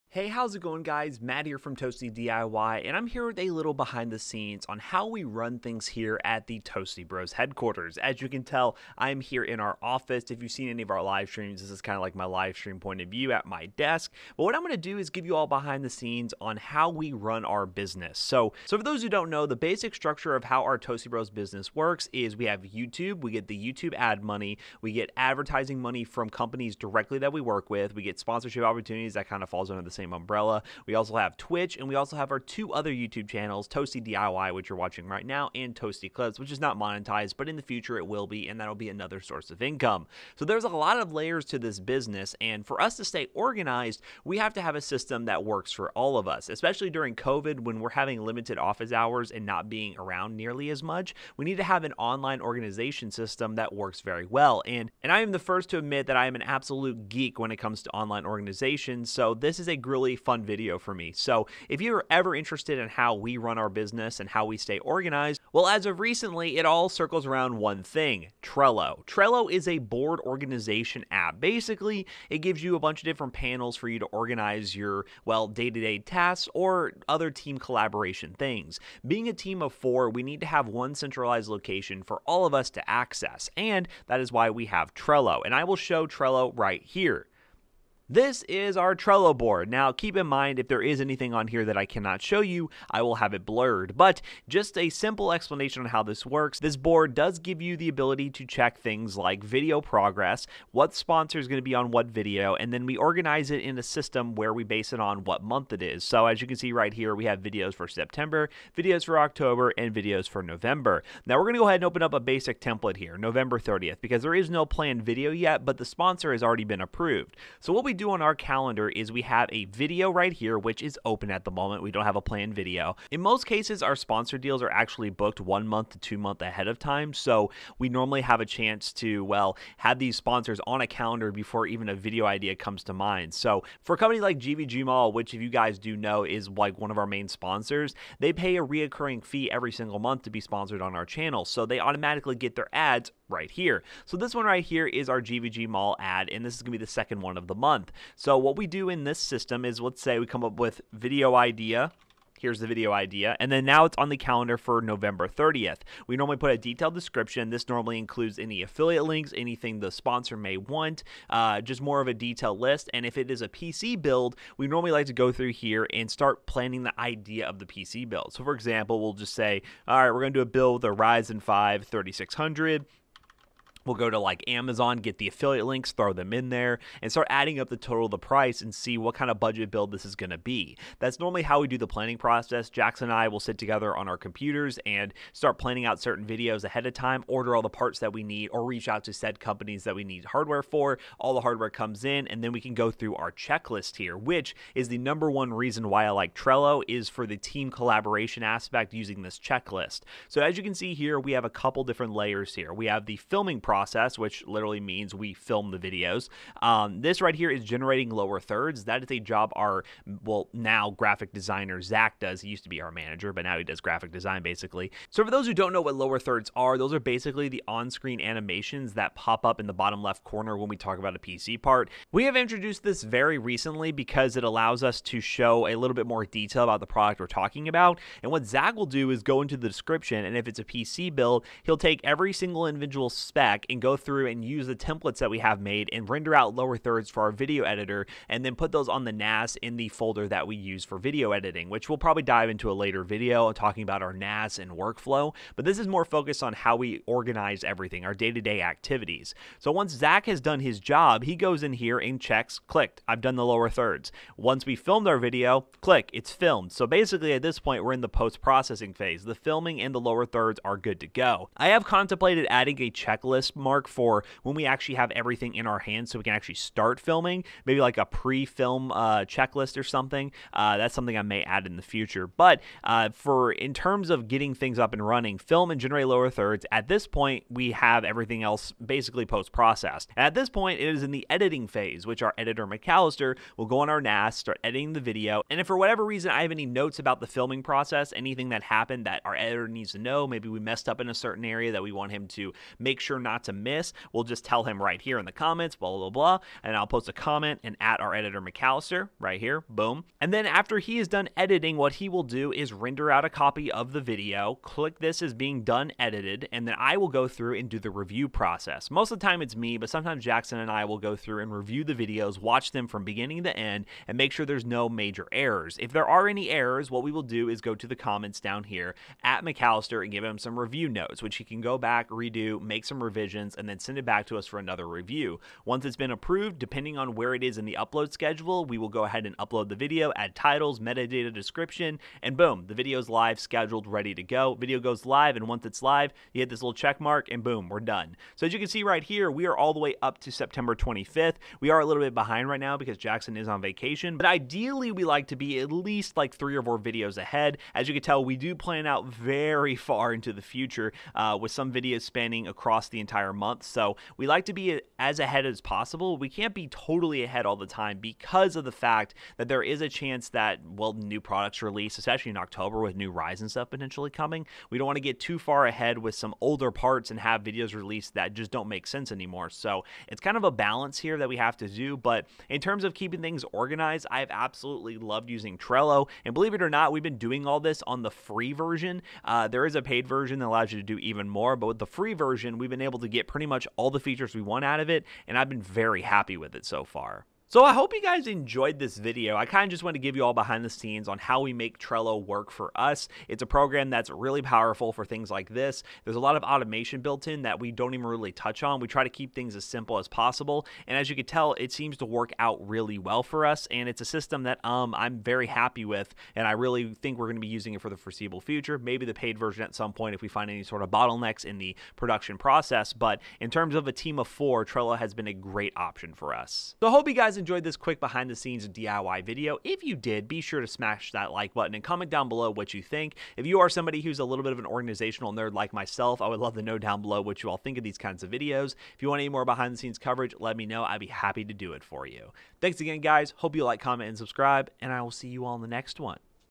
Trello 2020 Tutorial - How We Use Trello for YouTube + Workflow Example
As we expand the Toastybros brand, we have a lot of different projects to track and Trello has become our go to way to tracking and managing projects for our team. Trello is a free project management program that works great for small business like ourselves.